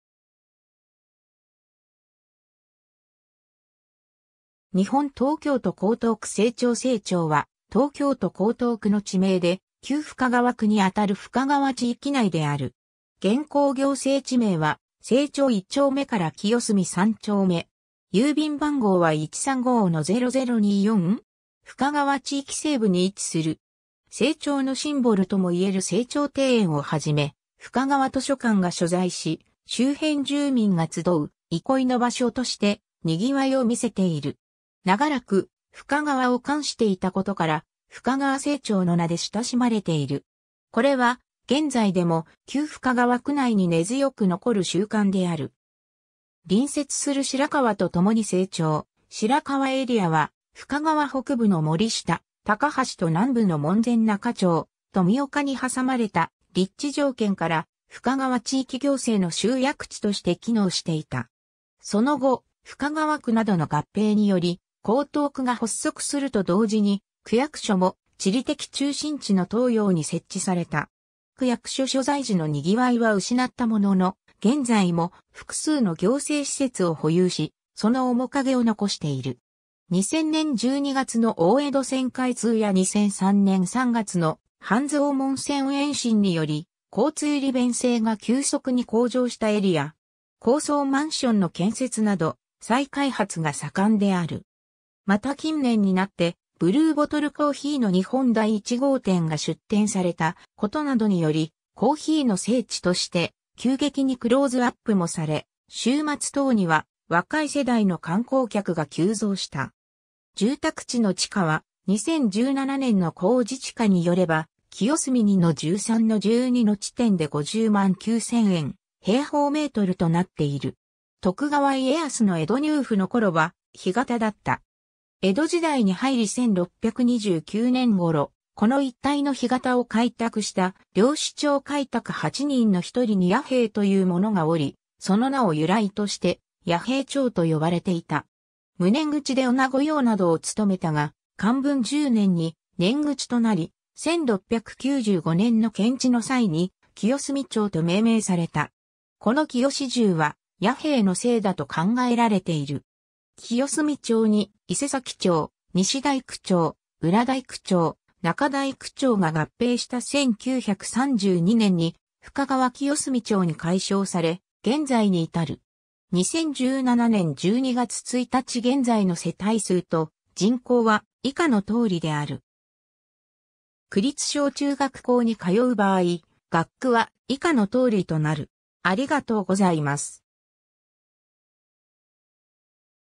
清澄 (江東区)
日本 」 東京都 」 江東区 」 清澄 清澄（きよすみ）は、東京都江東区の地名で、旧深川区に当たる深川地域内である。
現行行政地名は清澄一丁目から清澄三丁目。
 深川地域西部に位置する。
清澄のシンボルともいえる清澄庭園をはじめ、深川図書館が所在し周辺住民が集う憩いの場所として賑わいを見せている。
長らく深川を冠していたことから「深川清澄」の名で親しまれている。
これは、現在でも旧深川区内に根強く残る習慣である。
 隣接する白河と共に清澄・白河エリアは、深川北部の森下・高橋と南部の門前仲町・富岡に挟まれた立地条件から、深川地域行政の集約地として機能していた。
その後、深川区などの合併により江東区が発足すると同時に、区役所も地理的中心地の東陽に設置された。
区役所所在時の賑わいは失ったものの、現在も複数の行政施設を保有し、その面影を残している。
 2000年12月の大江戸線開通や2003年3月の半蔵門線延伸により、交通利便性が急速に向上したエリア。
高層マンションの建設など再開発が盛んである。
また近年になってブルーボトルコーヒーの日本第一号店が出店されたことなどにより「コーヒーの聖地」として急激にクローズアップもされ、週末等には若い世代の観光客が急増した。
 住宅地の地価は、2017年（平成29年）の公示地価によれば、清澄2-13-12の地点で50万9000円/m2となっている。
 徳川家康の江戸入府の頃は干潟だった。
江戸時代に入り1629年（寛永6年）頃、この一帯の干潟を開拓した猟師町開拓八人の一人に弥兵衛という者がおり、その名を由来として弥兵衛町と呼ばれていた。
無年貢地で御菜御用などを務めたが、寛文10年に年貢地となり、1695年（元禄8年）の検地の際に清住町と命名された。
この清住は弥兵衛の姓だと考えられている。
清住町に伊勢崎町・西大工町・裏大工町・仲大工町が合併した1932年（昭和7年）に深川清澄町に改称され、現在に至る。
 2017年（平成29年）12月1日現在の世帯数と人口は以下の通りである。